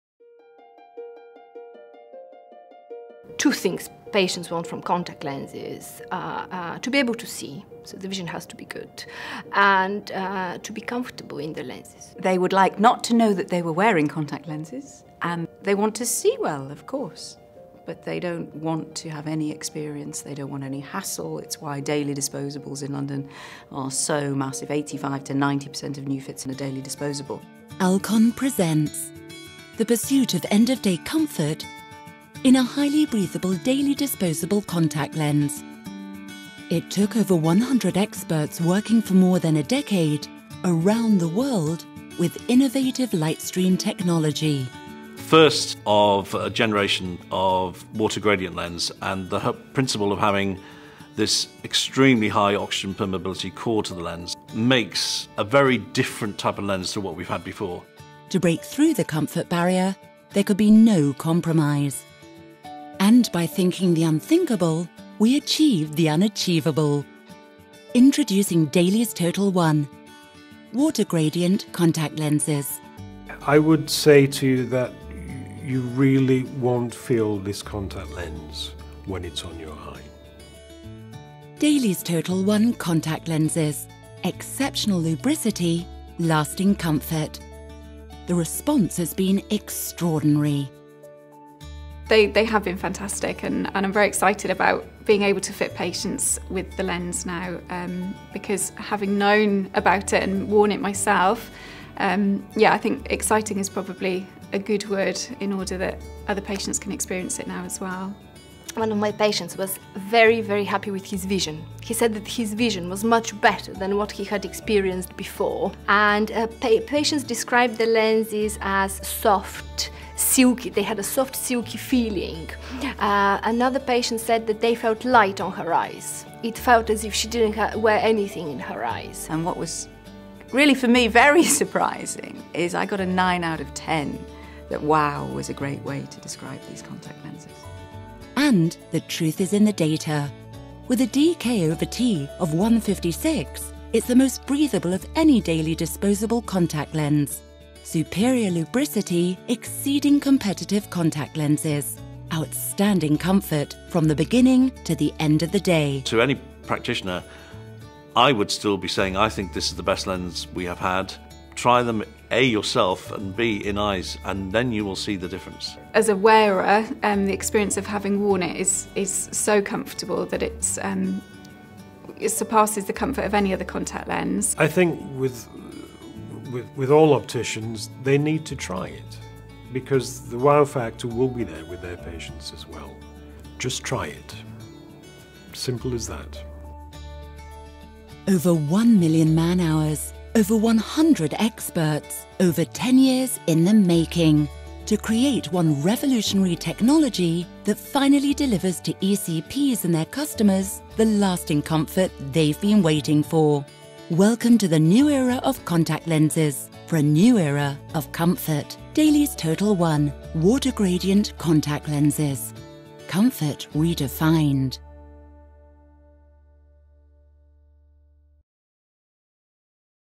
DAILIES TOTAL1 Water Gradient Contact Lenses - UK practitioner feedback
Amazing feedback from UK eye care practitioners who are delighted with the latest contact lens from Alcon - DAILIES TOTAL1 Water Gradient Contact Lenses

DAILIES TOTAL1 is the worlds first Water Gradient Contact Lens featuring an increase in water content from 33% at the core to over 80% at the surface. The core of the lens consists of a new silicone hydrogel polymer that has low water content and high oxygen transmissibility. The surface of the lens is composed of a network of hydrophillic polymers which have high a high water content of over 80%. At the very outer surface the water content approaches 100% to deliver outstanding comfort and lubricity.